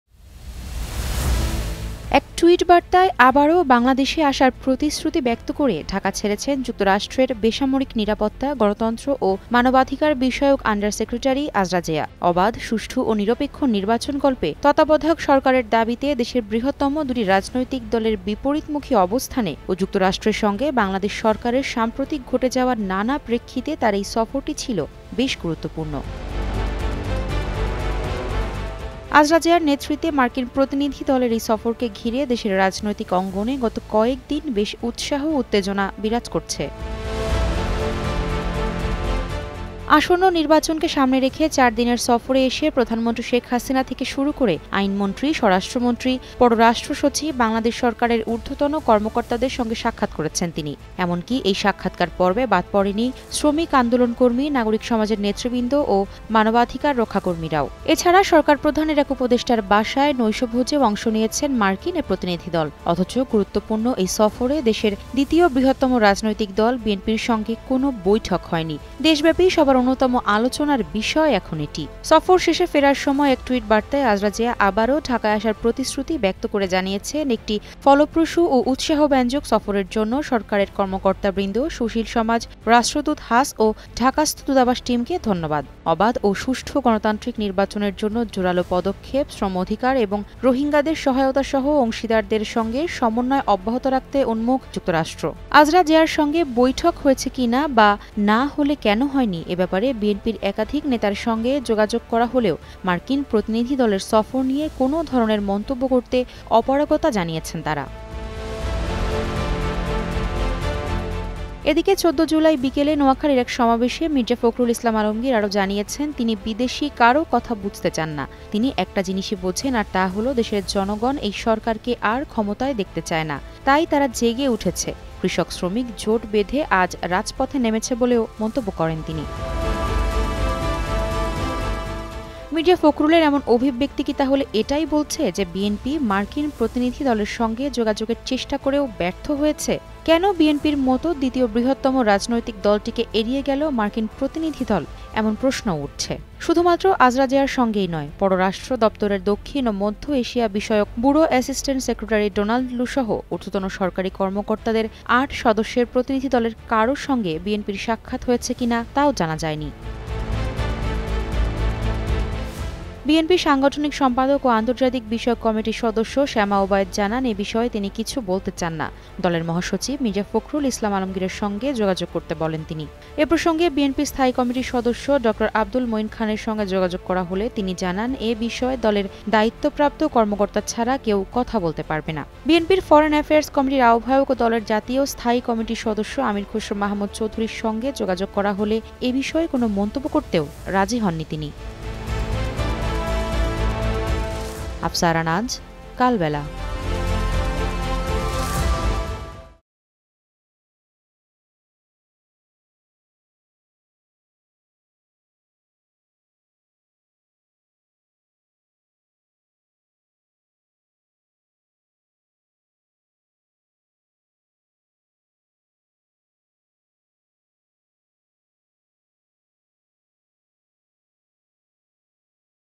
কেন আজরা জেয়ার দেখা পেল না বিএনপি | Uzra Zeya | BNP | USA | Kalbela
এক টুইট বার্তায় আবারও ঢাকায় আসার প্রতিশ্রুতি ব্যক্ত করে ঢাকা ছেড়েছেন যুক্তরাষ্ট্রের বেসামরিক নিরাপত্তা, গণতন্ত্র ও মানবাধিকারবিষয়ক আন্ডার সেক্রেটারি আজরা জেয়া। অবাধ, সুষ্ঠু ও নিরপেক্ষ নির্বাচনকল্পে তত্ত্বাবধায়ক সরকারের দাবিতে দেশের বৃহত্তম দুটি রাজনৈতিক দলের বিপরীতমুখী অবস্থানে ও যুক্তরাষ্ট্রের সঙ্গে বাংলাদেশ সরকারের সাম্প্রতিক ঘটে যাওয়া নানা প্রেক্ষিতে তার এই সফরটি ছিল বেশ তাৎপর্যপূর্ণ। কেন আজরা জেয়ার দেখা পেল না বিএনপি | Uzra Zeya | BNP |  USA | Kalbela